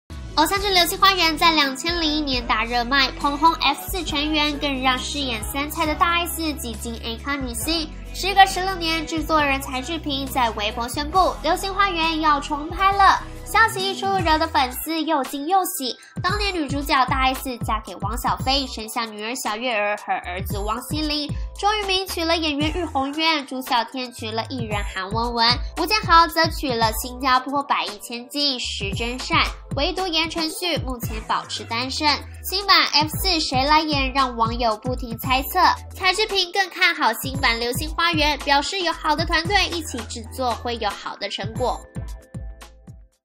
【3年前】時隔16年《流星花園》要重拍！ 當年主角都去哪了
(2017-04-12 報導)

偶像劇《流星花園》在2001年大熱賣，捧紅F4成員，更讓飾演杉菜的大S擠進A咖女星，時隔16年，製作人柴智屏在微博宣佈「《流星花園》要重拍了！ 」
消息一出惹的粉絲有驚又喜，當年女主角大S嫁給汪小菲，生下女兒小玥兒和兒子汪希箖，周渝民娶了演員喻虹淵，朱孝天娶了藝人韓雯雯，吳建豪則娶了新加坡百億千金石貞善，唯獨言承旭目前保持單身。
新版的F4誰來演讓網友不停猜測，柴智屏更看好新版《流星花園》表示「有好的團隊一起製作，會有好的成果」